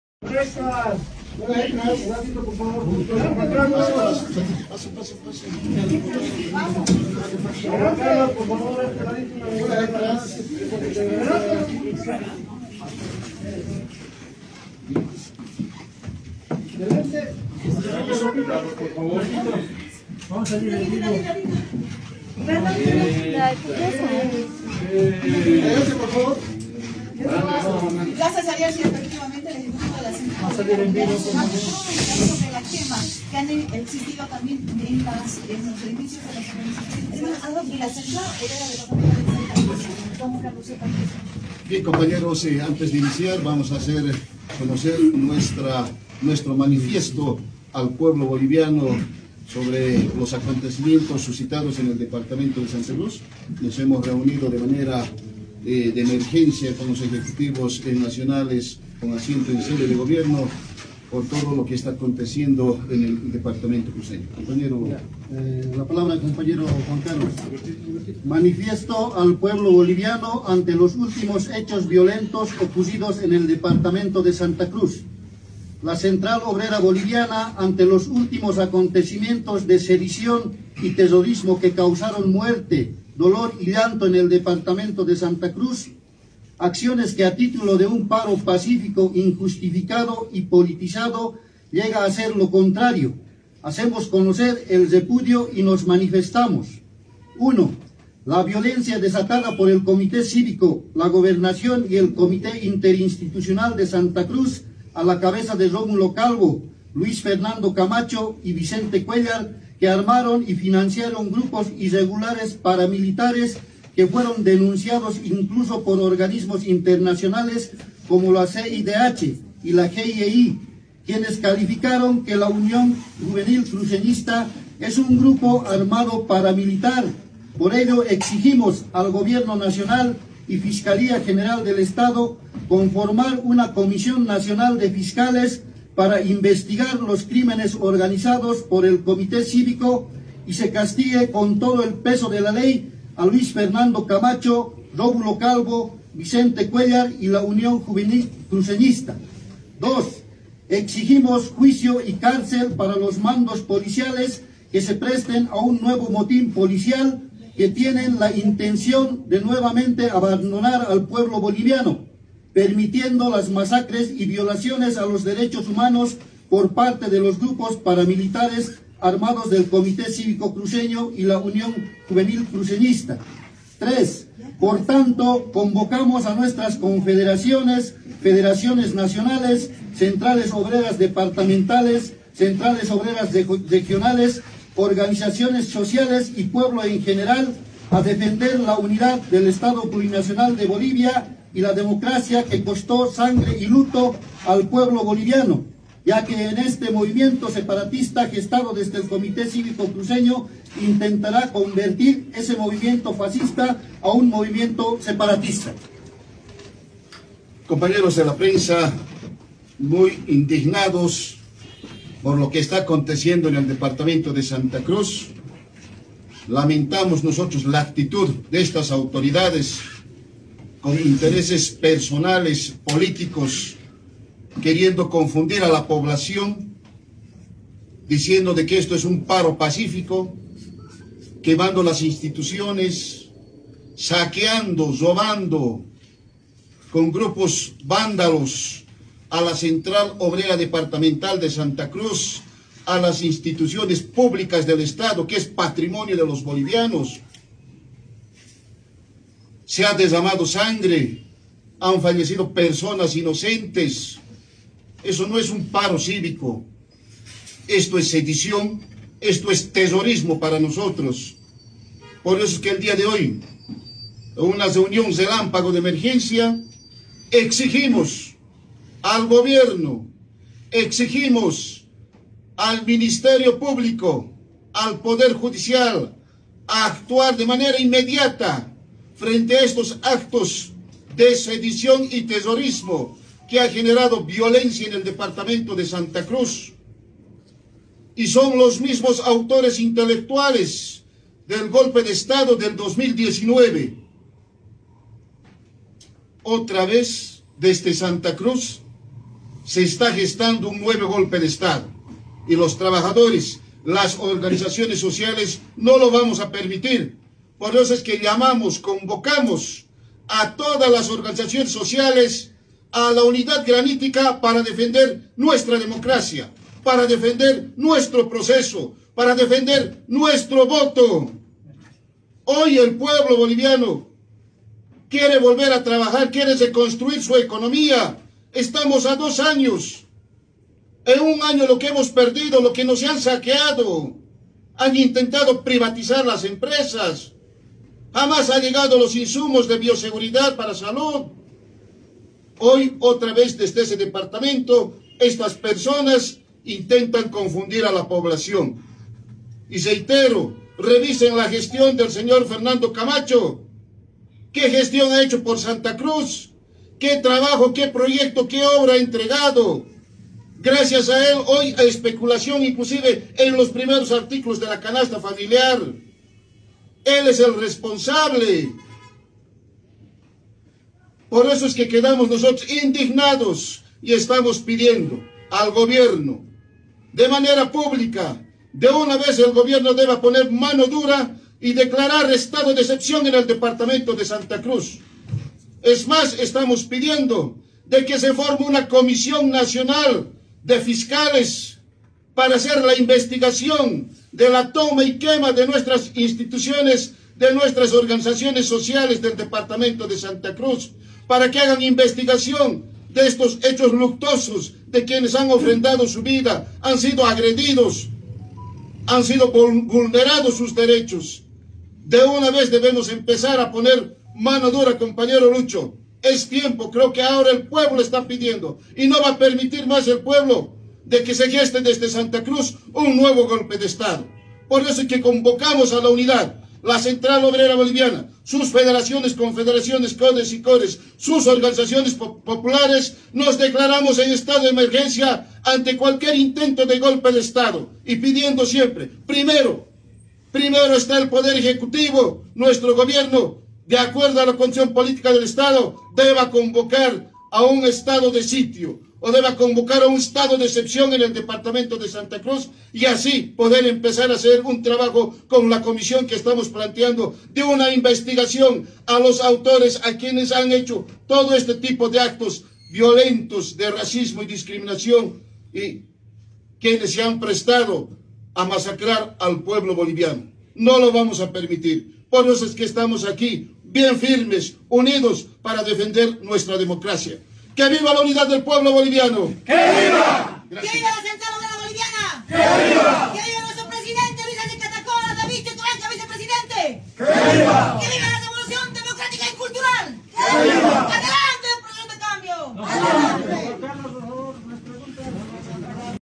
🔴 La COB demanda ‘mano dura’ a Arce para que no se geste otro ‘golpe’ desde Santa Cruz
**⭐ Damos la Bienvenida a Ben Grageda  ⭐ **
El ejecutivo de la COB, Juan Carlos Huarachi, pidió que el Presidente declare un estado de excepción en Santa Cruz, que se conforme una comisión de fiscales, y determinó el estado de emergencia en su organización.